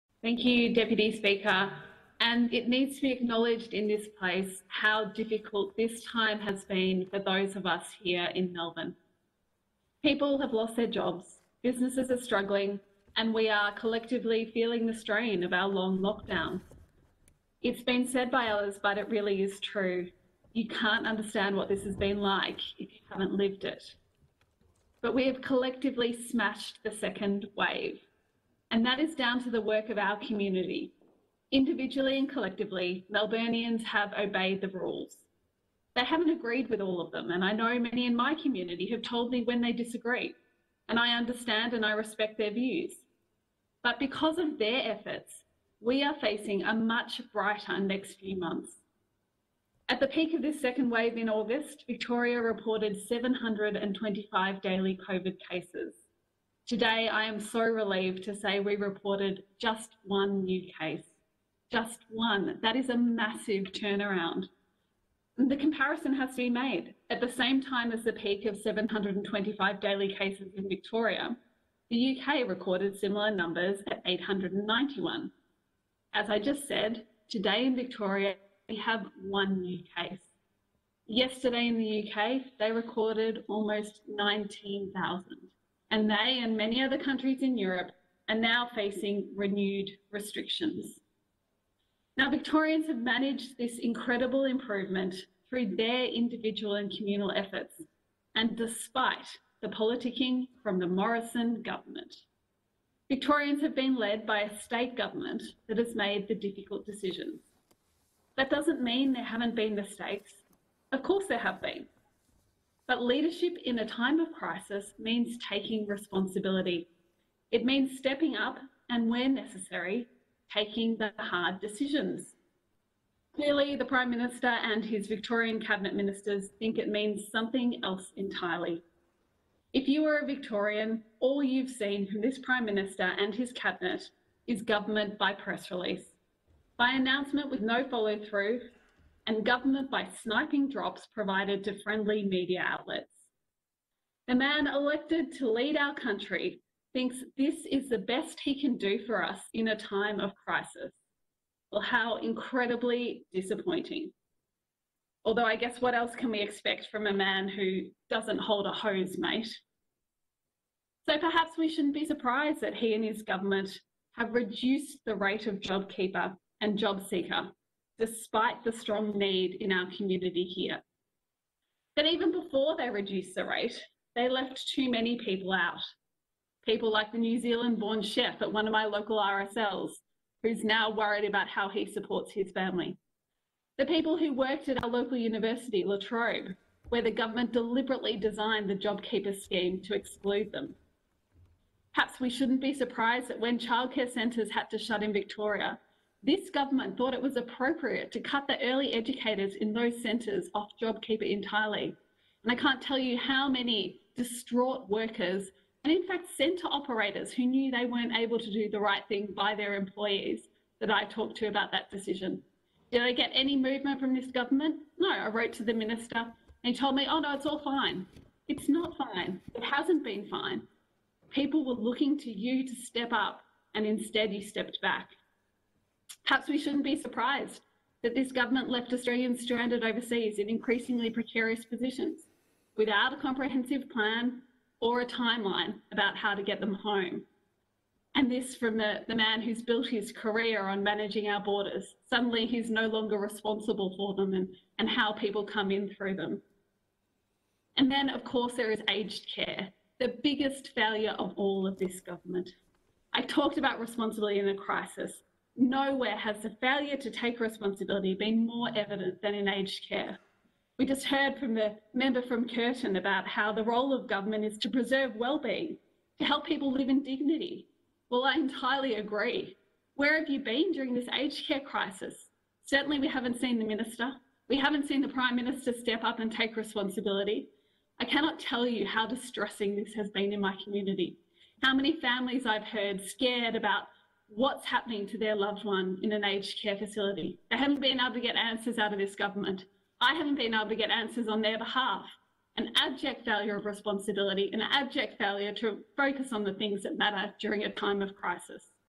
The Government’s inability to focus on the issues that matter during COVID-19.
The Government’s partisan attacks on State Governments are galling given their own tragic failures during the COVID-19 pandemic.
The Government should not be playing politics when they should be focusing on the crisis in age care facilities, when they are failing early childhood educators and reducing federal government payments; JobSeeker and JobKeeper.